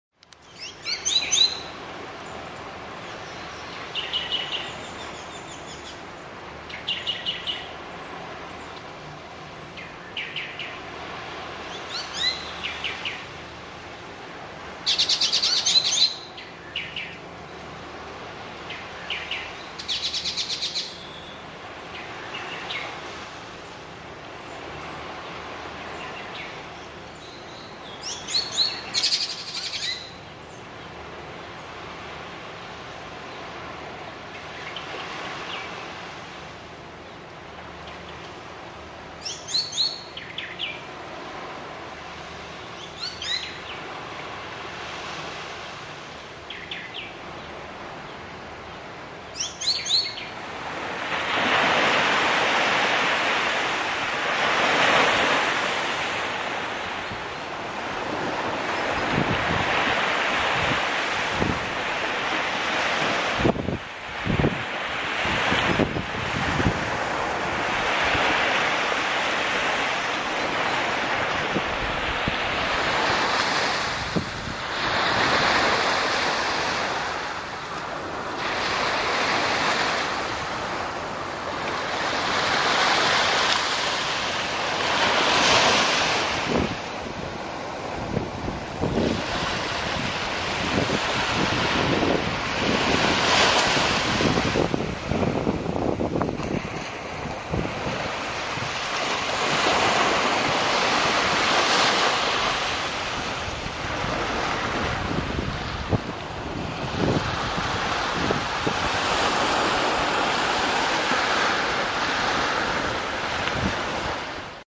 El Yaque, sunrise
Sounds of nature at El Yaque, Margarita island, Venezuela